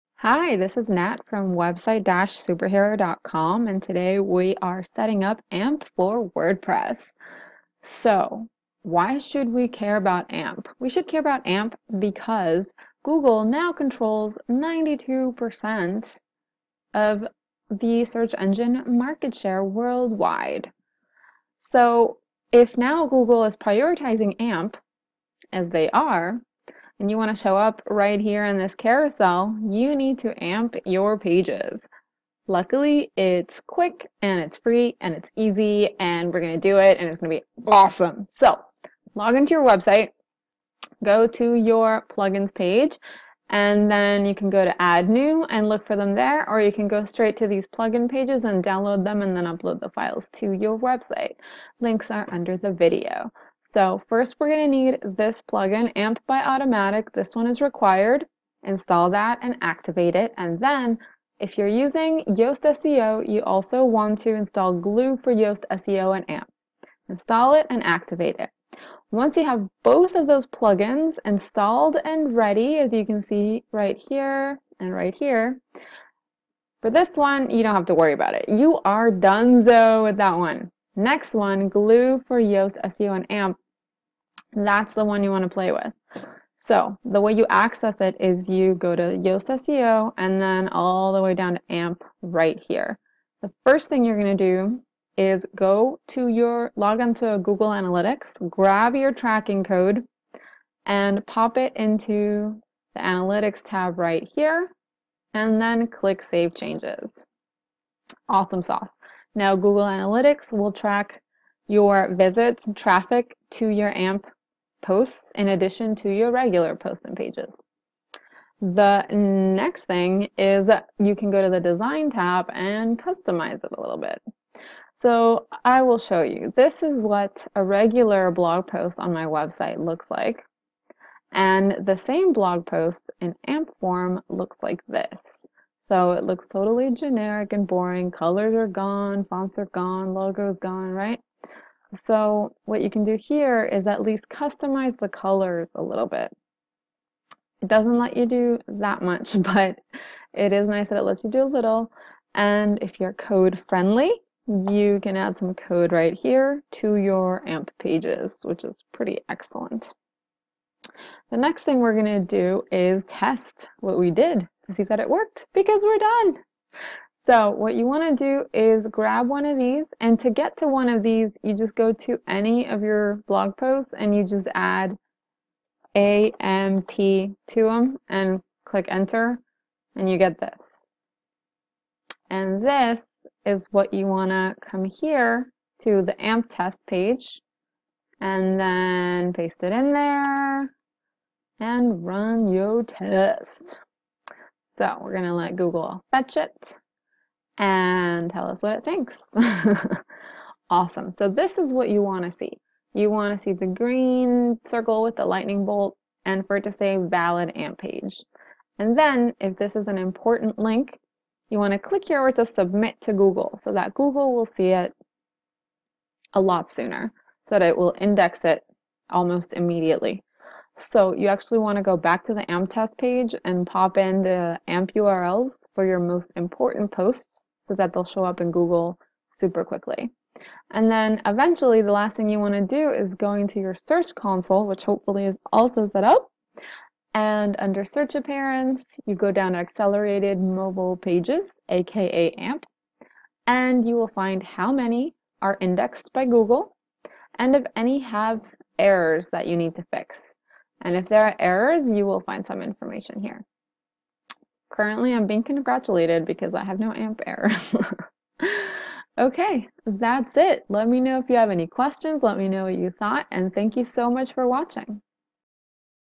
How to Set Up AMP for WordPress in 3 Minutes
Follow along step by step as we set up AMP for WordPress and test it in just 3 minutes!

It's easy, quick, and free to set up AMP for WordPress -- you'll elevate your SEO standing and make your website better friends with Google! This means people will find you more easily and you will grow your traffic, readership, and hopefully your client base with less effort.

Links mentioned in the video that you'll use to set up AMP for WordPress:

Read the full post on how to set up AMP for WordPress here on the Website Superhero blog Click over to the post so you don't miss any details!